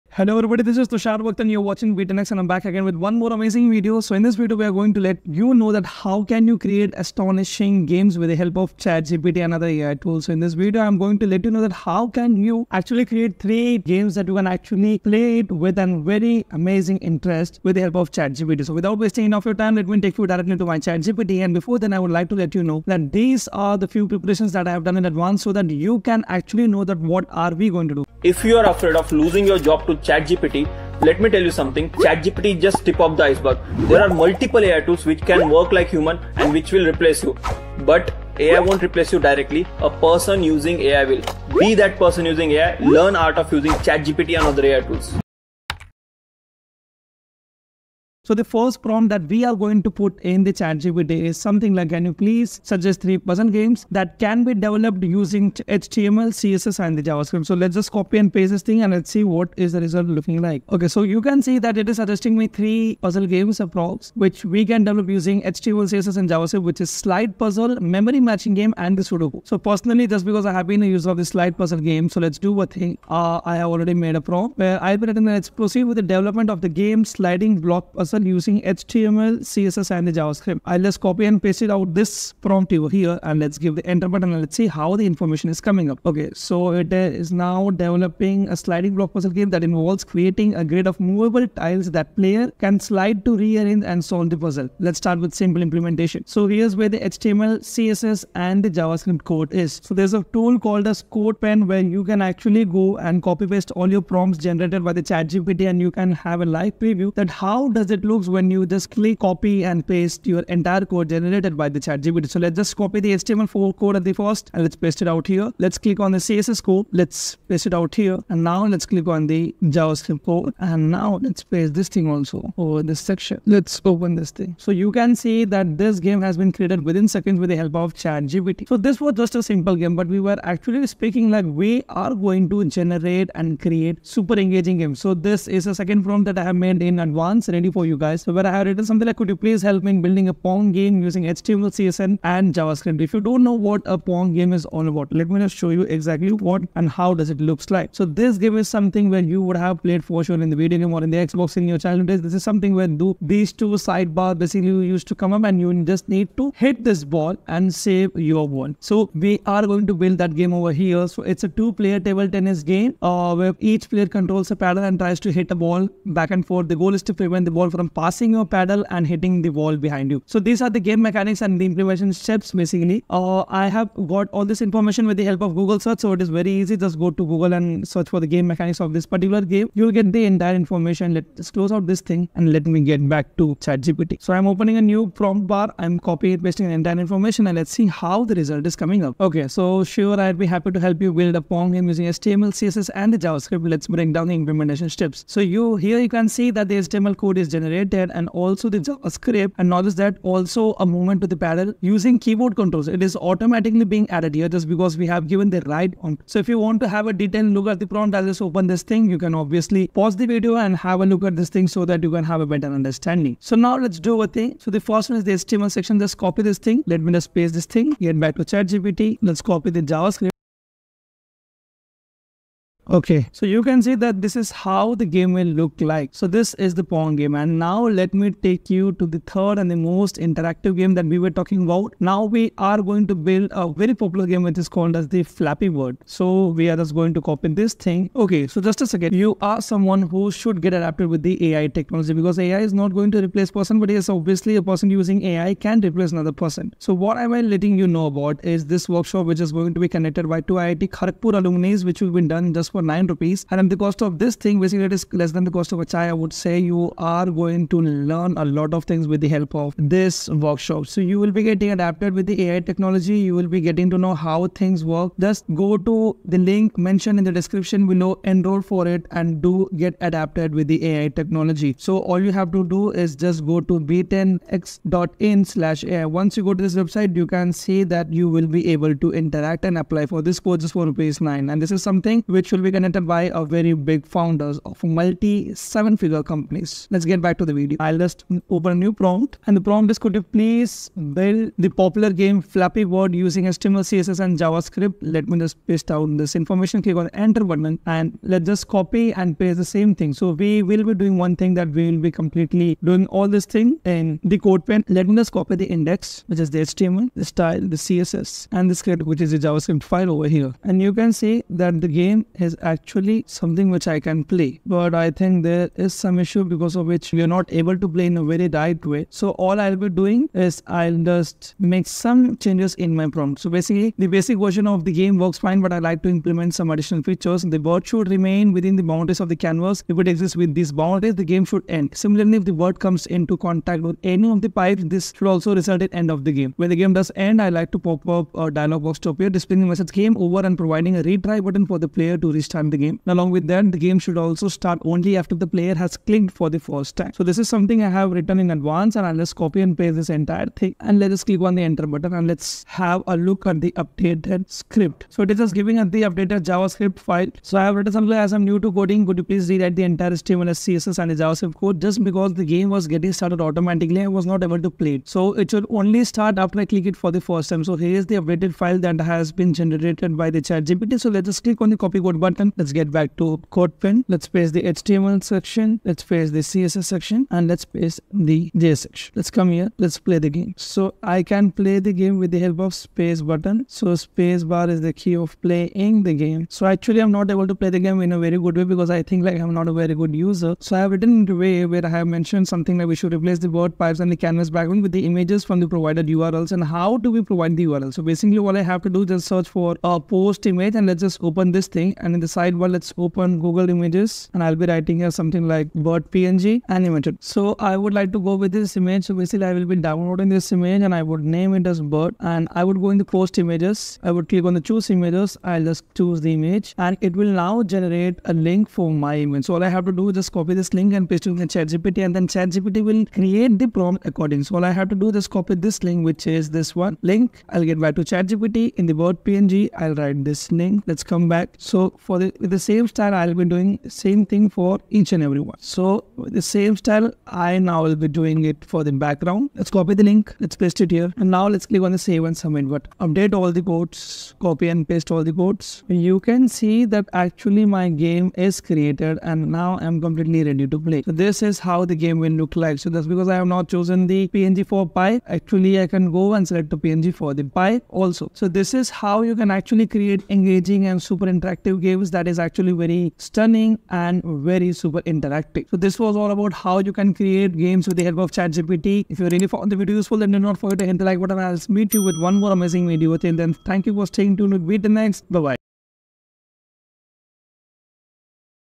Master Game Development with ChatGPT: Create HTML/CSS/JavaScript Games Easily! Be10x
Master Game Development with ChatGPT: Create HTML/CSS/JavaScript Games Easily!
Welcome to another exciting video of Be10x where we guide you through the fascinating world of game development using ChatGPT and other AI tools. In this tutorial, we delve into creating three captivating games: a sliding block puzzle, a classic pong game, and the ever-popular Flappy Bird, all using HTML, CSS, and JavaScript with the help of AI.

🔗 *Elevate your career with AI. Join Be10x's 3-hour workshop for advanced AI tool mastery. Limited spots – Secure yours now!* -
Key Highlights:

- Developing a Sliding Block Puzzle: Learn how to create this intriguing puzzle game. We walk you through the process, from generating the code using ChatGPT to implementing it in CodePen for a live preview.

- Building a Pong Game: Discover how to recreate this nostalgic two-player table tennis game. We provide insights into the game mechanics and step-by-step coding instructions.

- Creating Flappy Bird: Step into the world of this addictive game. We show you how to build it from scratch, including customizations and gameplay enhancements.
Additional Insights:

- Understanding the potential of AI in game development.
- Practical demonstrations using ChatGPT for coding.
- Tips for beginners in coding and game development.
- Interactive and easy-to-follow instructions.
Conclusion:
This video is not just about game development; it's about embracing AI technology in creative ways. Whether you're a beginner or an enthusiast, these projects will enhance your skills and understanding of AI's role in modern programming.